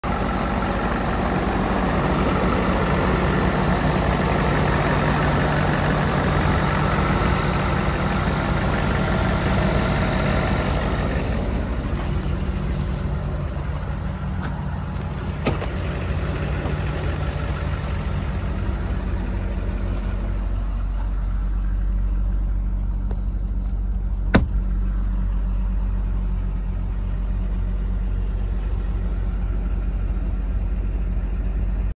LOT 297 • 2016 FORD F250XL SUPER DUTY 4X4
6.7L V8 DIESEL ENGINE, AUTOMATIC TRANSMISSION, 4 DOOR, CLOTH INTERIOR, 8FT BED, ODOMETER SHOWING 239,088 MILES, VIN 1FT7W2BT0GEC79639